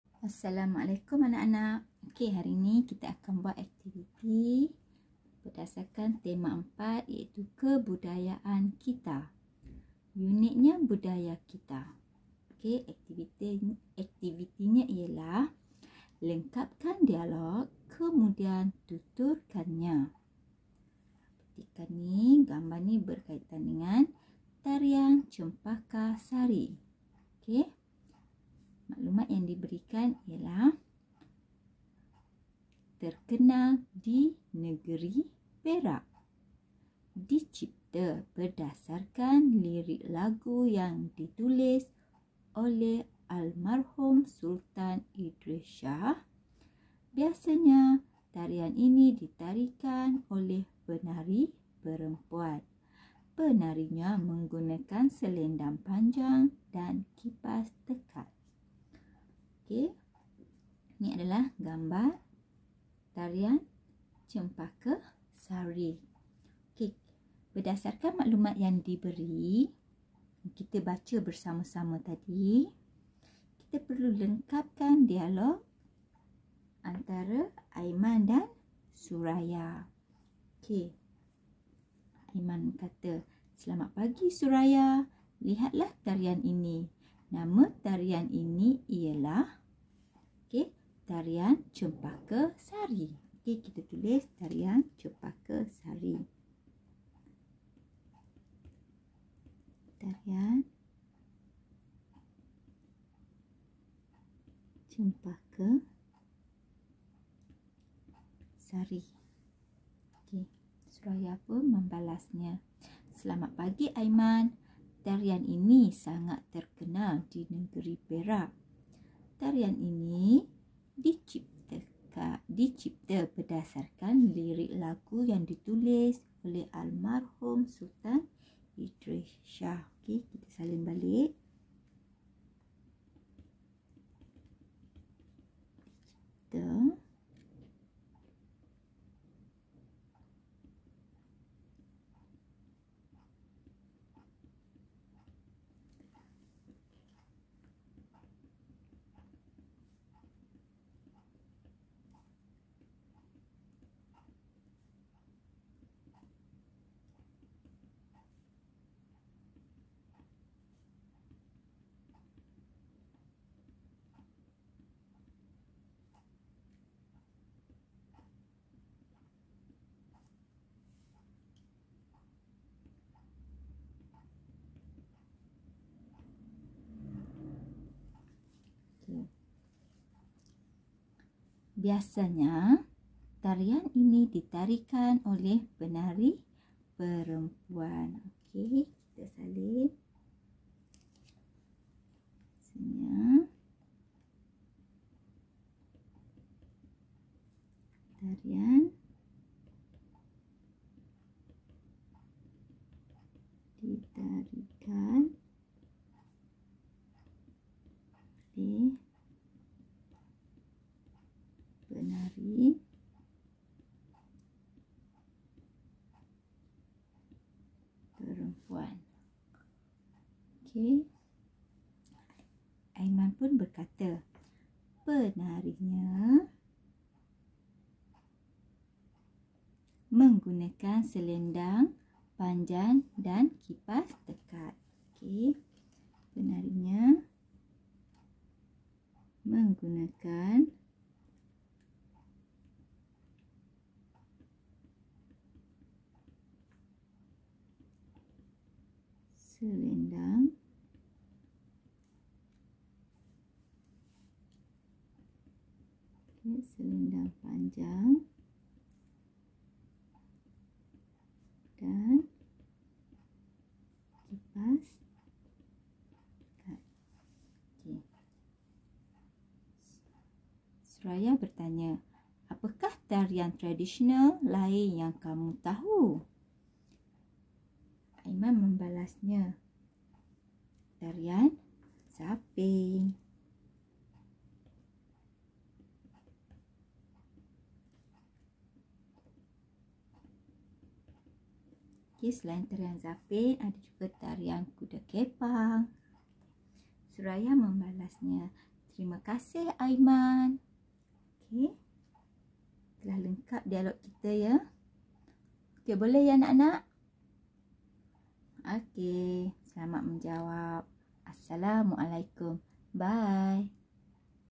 BUKU AKTIVITI BAHASA MELAYU TAHUN 2 JILID 1 MUKA SURAT 71 KPM
Jom menyiapkan kerja sekolah bersama Cikgu Rose Ina🌹